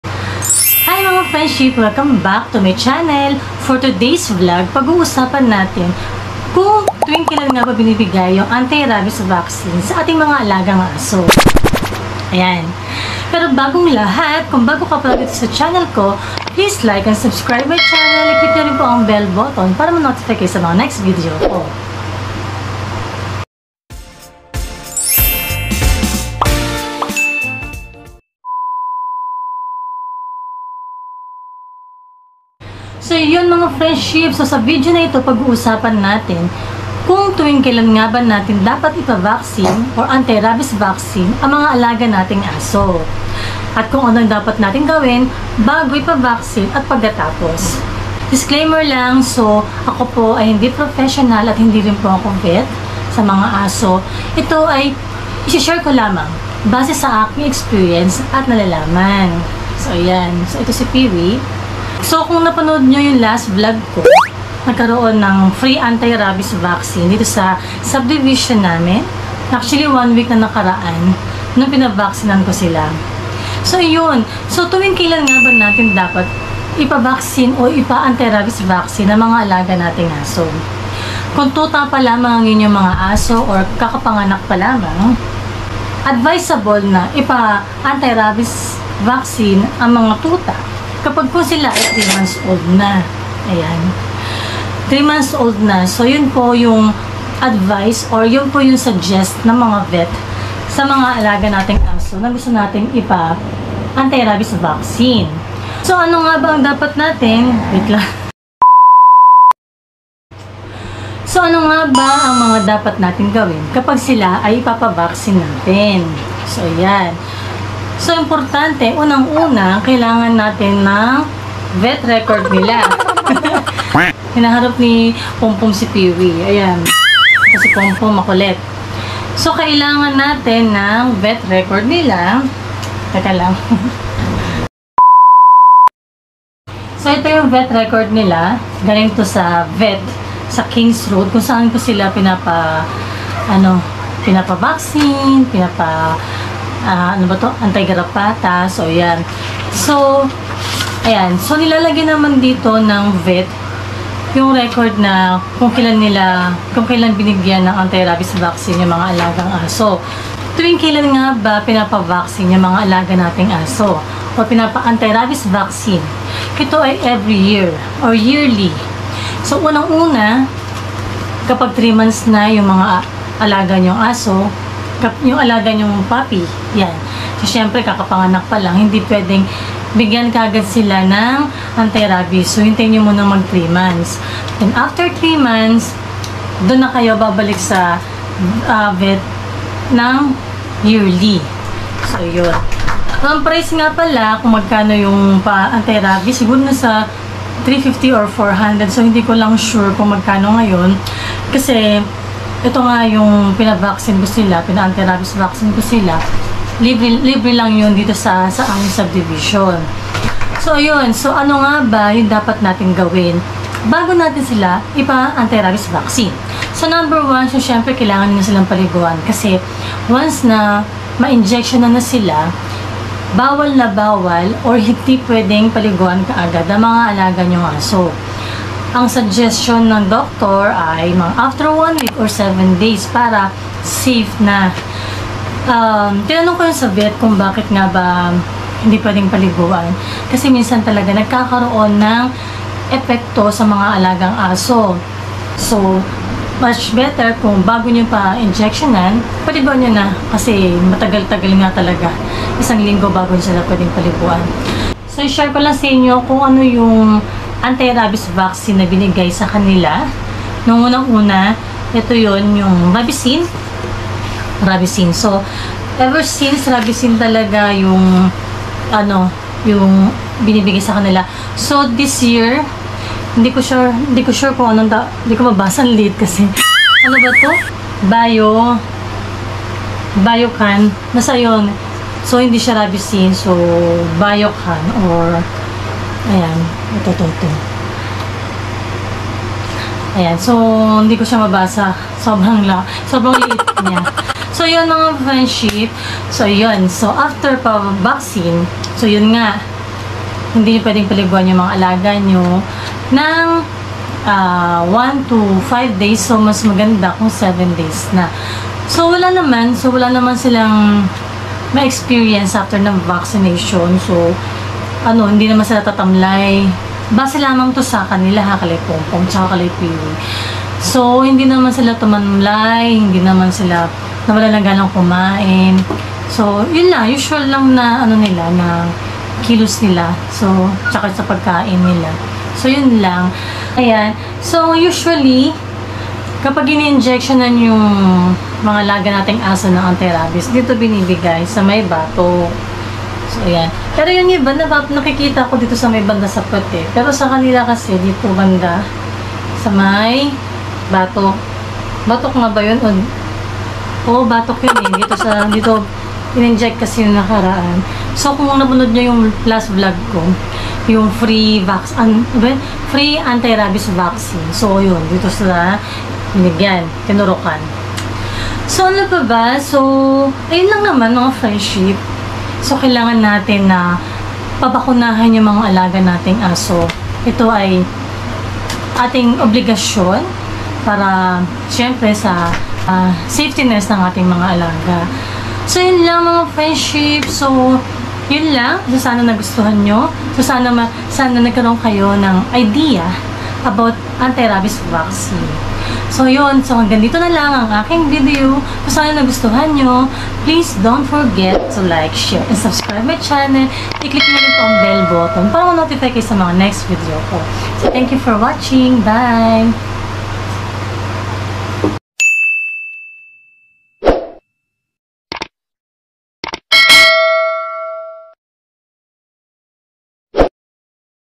Anti Rabies Vaccine sa Aso/ Tuwing kelan ba binibigay sa Aso?/ ShihTzu/ jenny apelo
Hi mga friendship sa mga furparents na nagtatanong tuwing kelan nga ba binibigay ang Anti Rabies Vaccine sa Aso.